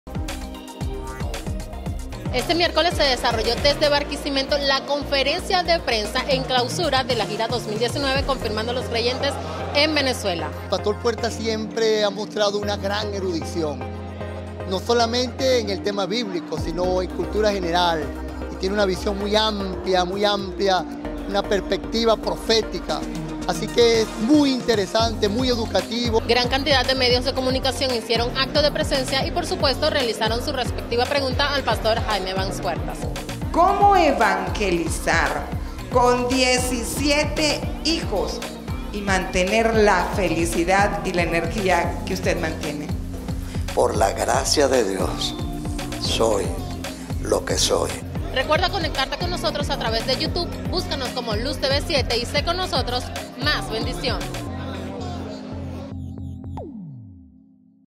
RESUMEN | RUEDA DE PRENSA | BARQUISIMETO | GIRA 2019 | AVANCE | REPORTAJE
Clausurando la gira2019 de Venezuela en Barquisimeto, se desarrollo la conferencia de prensa en el hotel hostería de la capital larense con la presencia de periodistas, medios de comunicación de demás.

El Hno Puertas, tras su llegada a la ciudad de Barquisimeto se reunió con los medios de comunicación, donde dio detalles de la gira en Lara y las fiestas a realizarse, respondió algunas preguntas de la actualidad, lo que pasa en Venezuela y dejó algunas recomendaciones a los asistentes...

La gira confirmando a los creyentes es el recorrido que realiza el hno. Puertas por diferentes ciudades de algunos países, predicando la palabra de Dios en concentraciones multitudinarias, Al llegar el Hno Puertas a cada ciudad, se realizan múltiples actividades compuesta por la reunión de pastores, fiesta de las dinamitas, fiesta de la juventud y fiesta de multitudes.

Reportera: Génesis Reyes.

Camarógrafo: @GersonDaboin
Gerson Daboin en instagram

Editor: @JuanMarquez
Juan Marquez en instagram